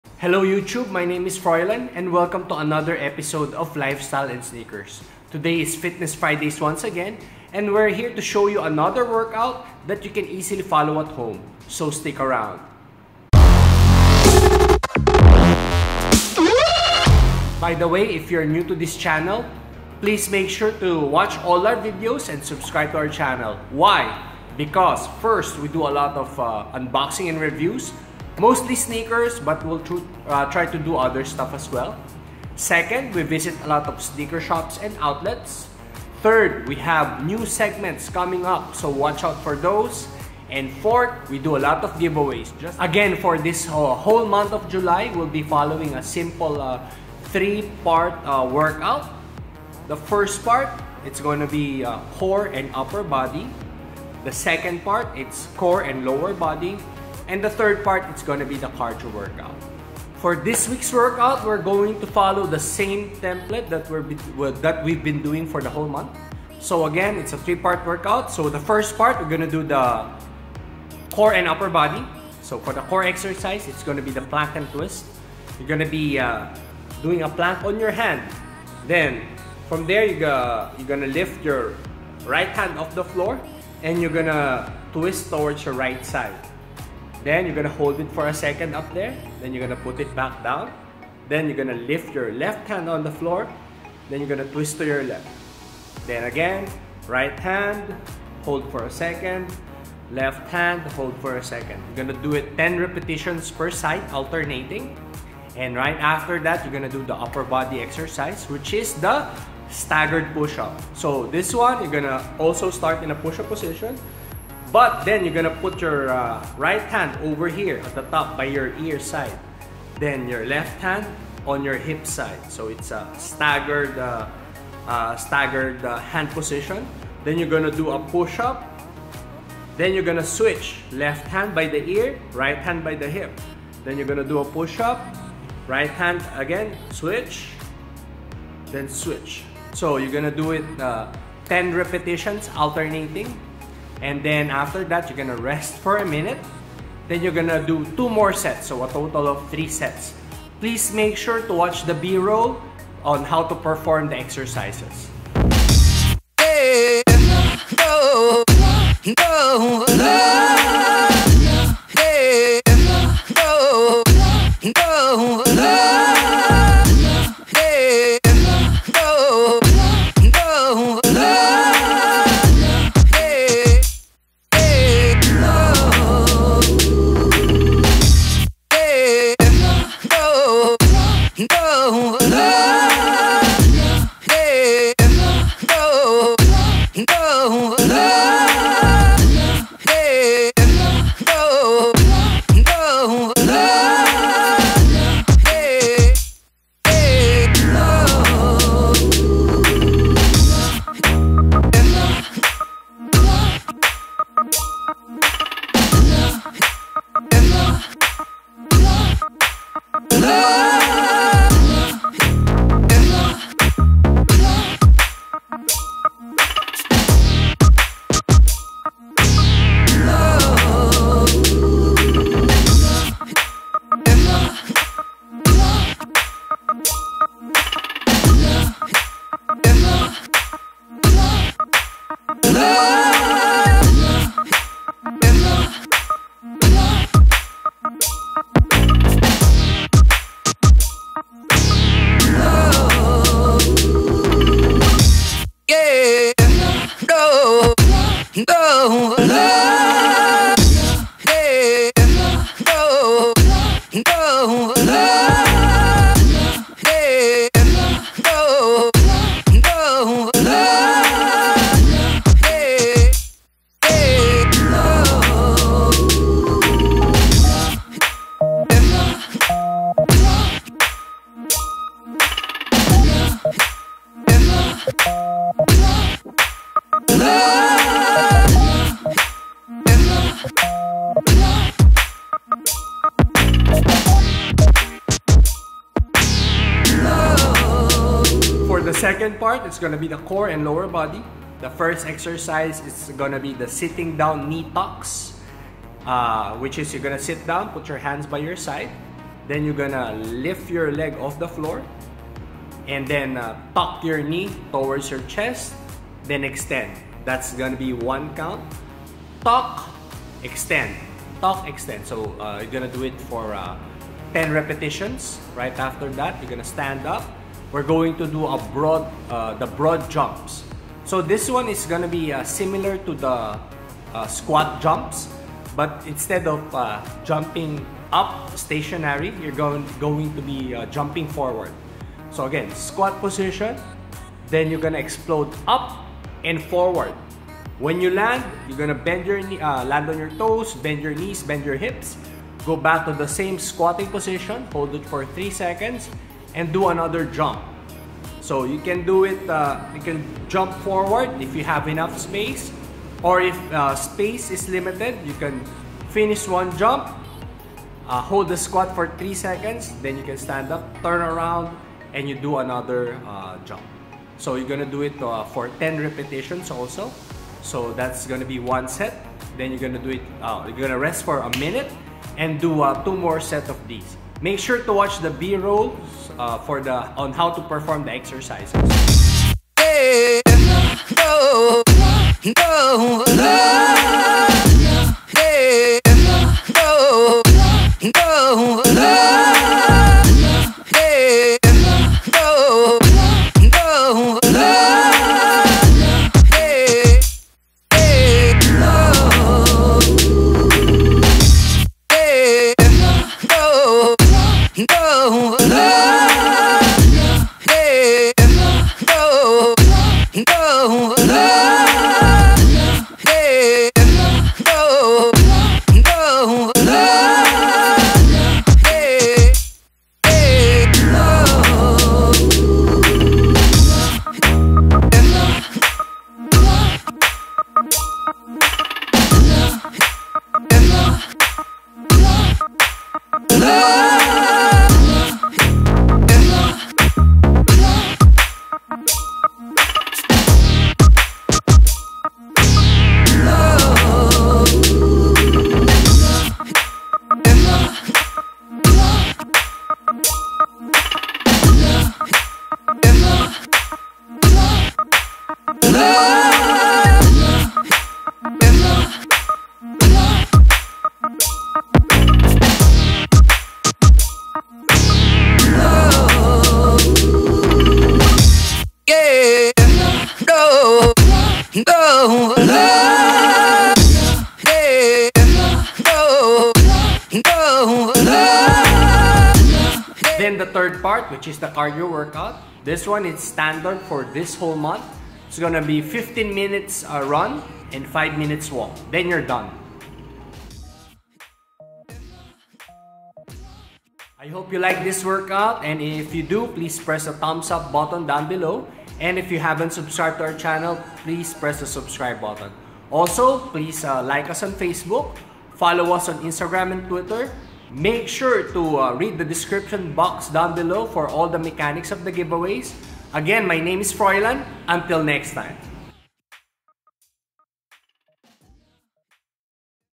Fitness Friday: Plank and Twist/Staggered Push ups and Sitting Knee Tucks/Broad Jumps
We have another Fitness Friday episode here at Lifestyle and Sneakers. This day were doing the Plank and Twist/ Staggered Push ups for the first set and for the second set we will do the Sitting Knee Tucks/Broad Jumps. The last set again we are doing a Cardio work out with 15 minute run and 5 minute walk. We hope you enjoy the work out!